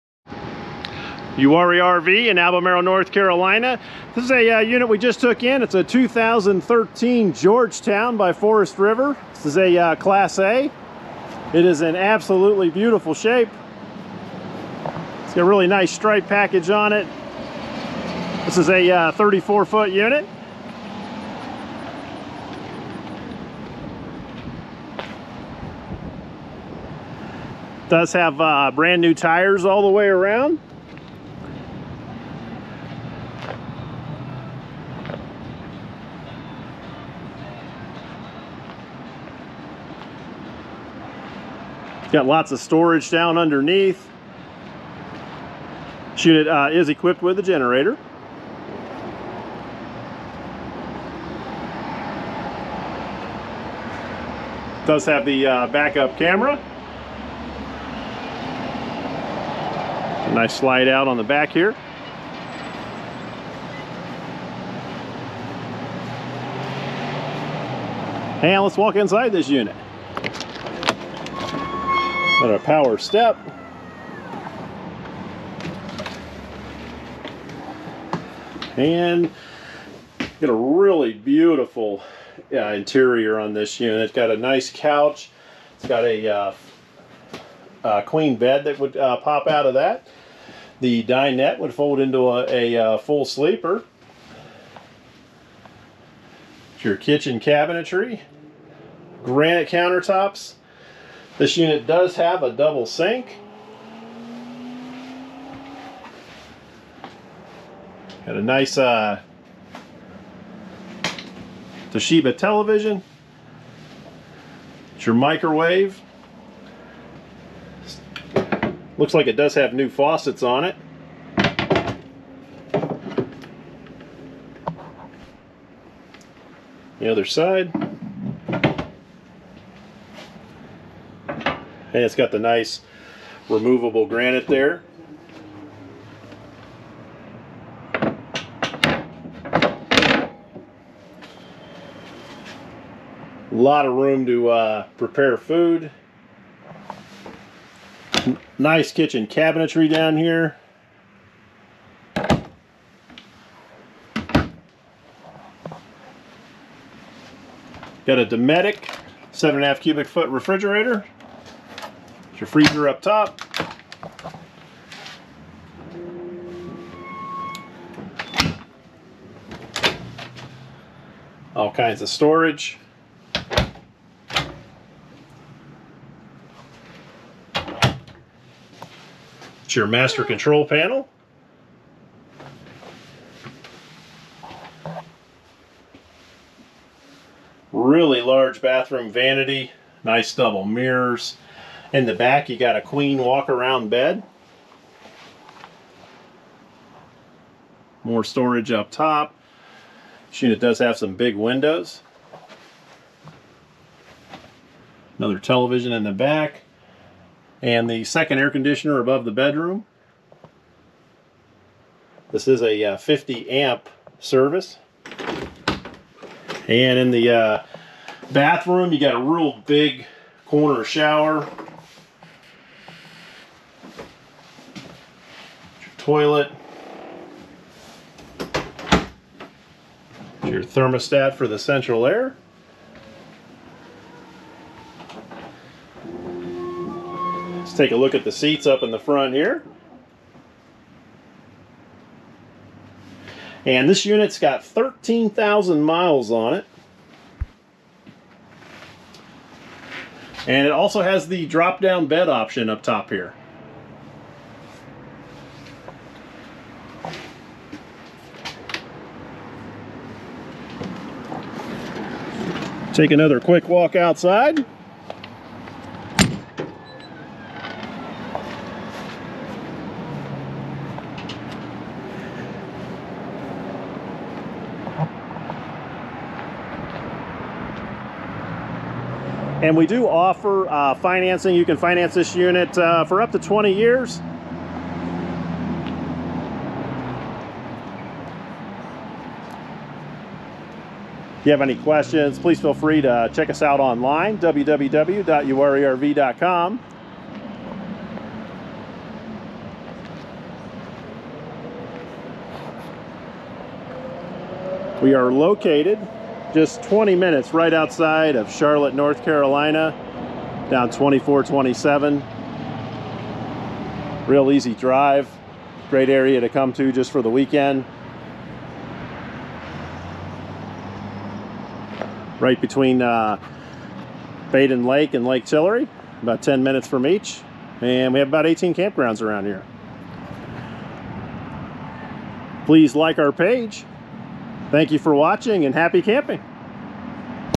2013 Forest River Georgetown 329DS Class A Motorhome
Local unit. Well cared for and maintained. Plenty of room. Great price.

Located 35 minutes east of Charlotte, NC in Albemarle, NC